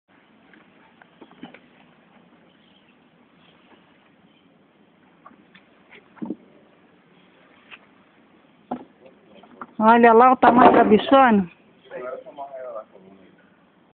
transplante de palmeira - moinho de vento
Esse é o vídeo mais legal, pois estávamos colocando ela no berço novo.